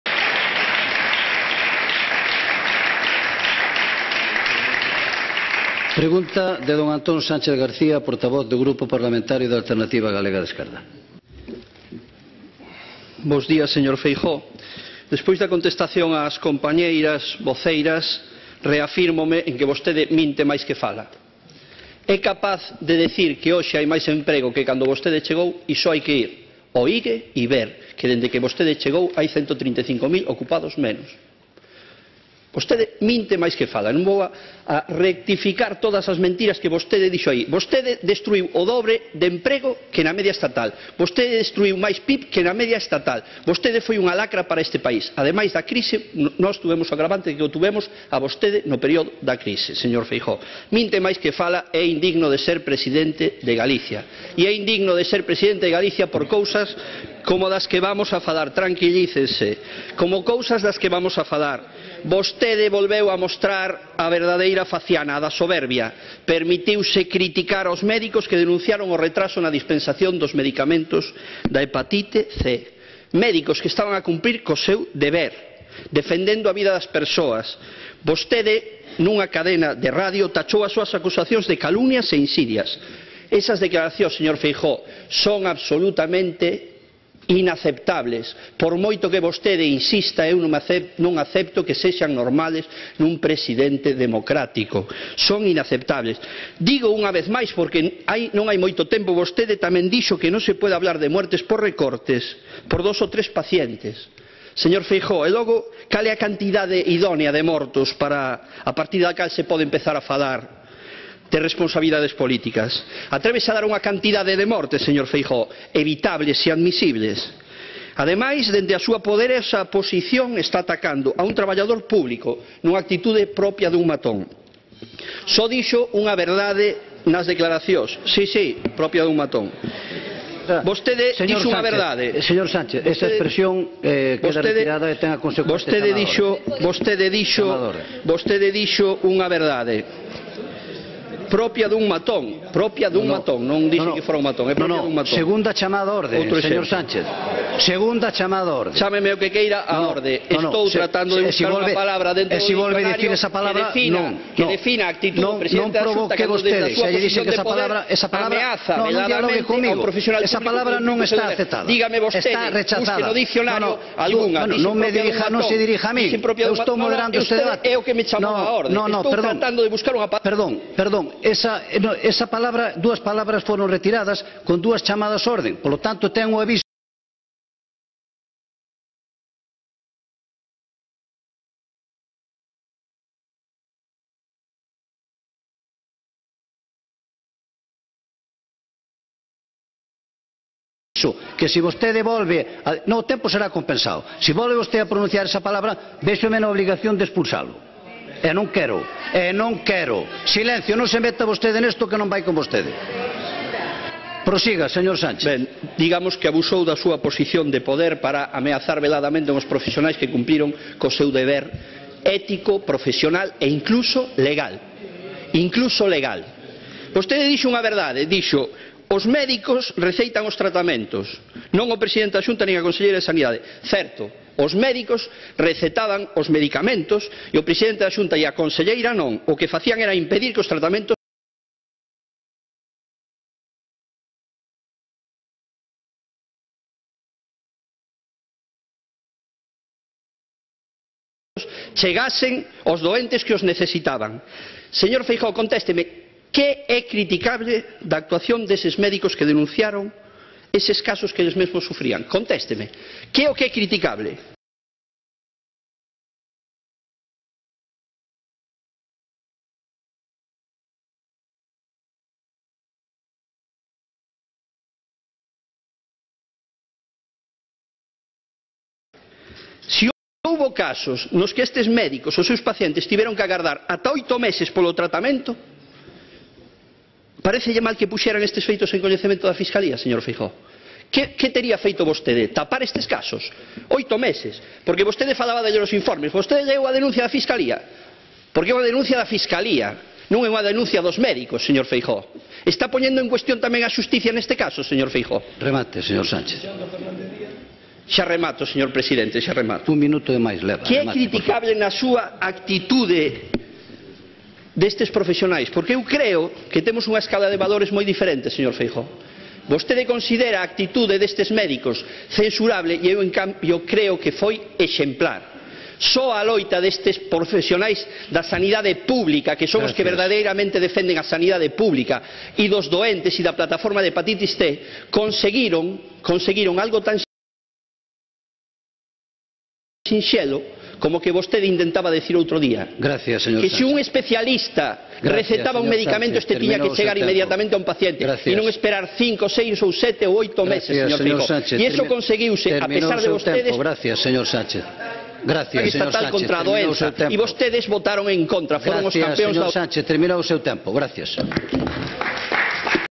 "Actuou coma un matón atacando aos médicos que denunciaron retrasos nos tratametnos de hepatite"
O viceportavoz de AGE, Antón Sánchez, centrou a sesión de control ao presidente Alberto Núñez Feijóo en denunciar as súas indignas declaracións nun medio de comunicación atacando aos médicos que denunciaron os retrasos na administración dos tratamentos a enfermos de hepatite C que morreron antes de recibir ese tratamento. Na súa intervención asegurou que a actitude do presidente da Xunta era propia dun "matón" o que provocou dúas chamadas á orde por parte do presidente da Cámara, Miguel Santalices, que chegou a amenazar coa expulsión do viceportavoz de AGE.